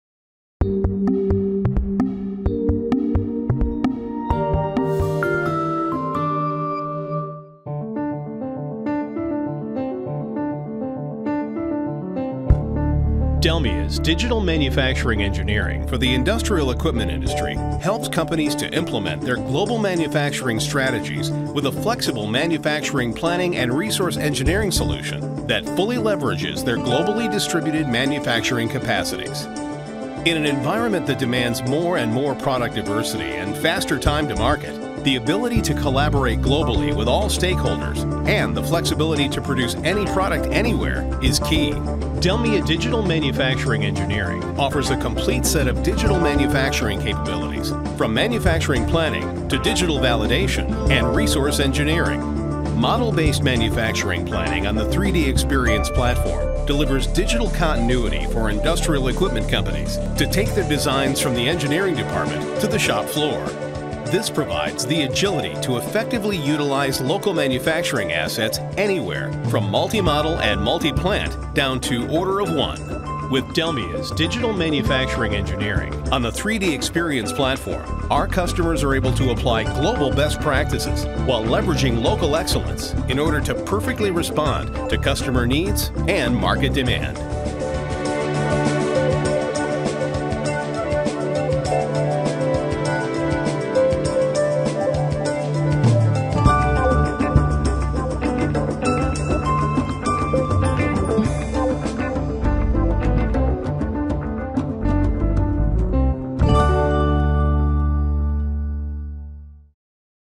Digital Manufacturing Engineering Software | DELMIA
DELMIA’s digital manufacturing engineering for Industrial Equipment helps companies implement their global manufacturing strategies with a flexible manufacturing planning and re scheduling resource engineering solutions that fully leverages their global manufacturing capabilities. Goals include increasing local effectiveness of global manufacturing strategies, mastering product & production diversity and implementing seamless digital continuity.